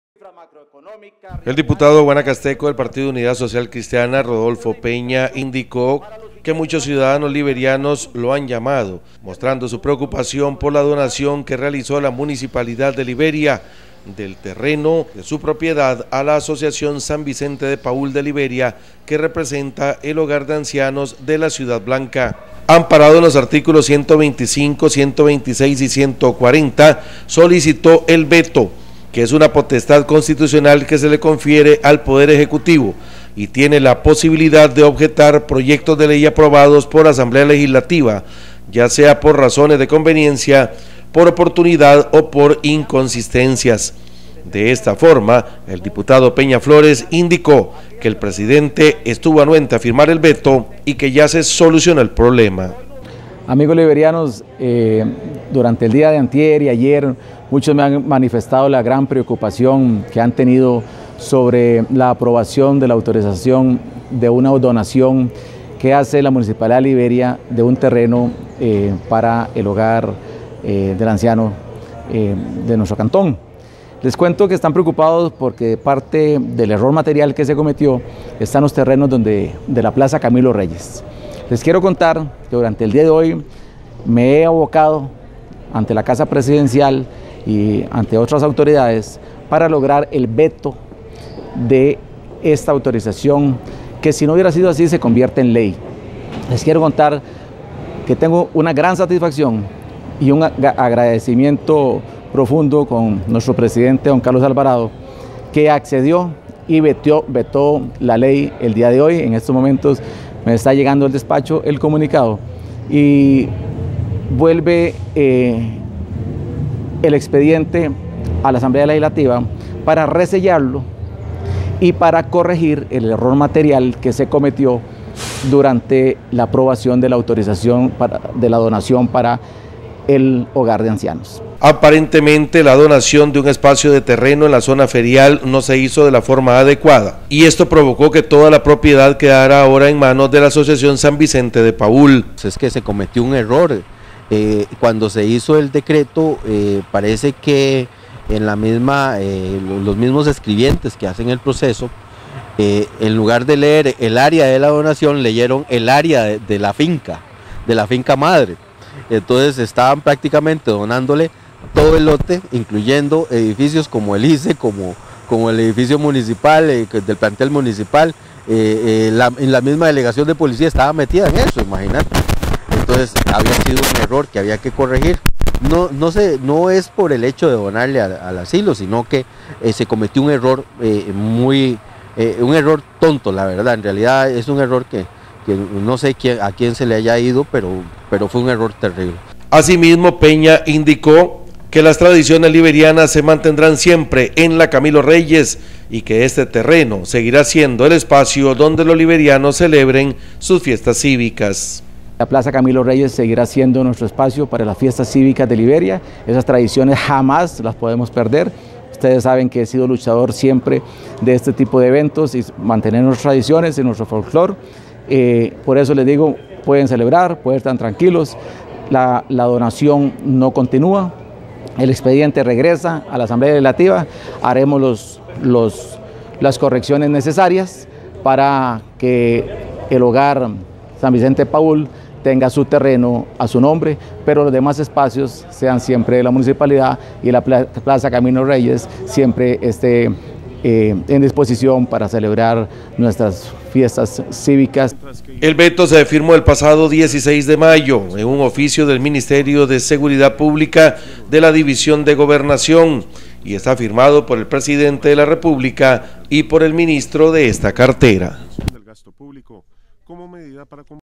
Diputado Rodolfo Peña logra veto a decreto que limitaba espacios comunales para los liberianos
Nota Anexión TV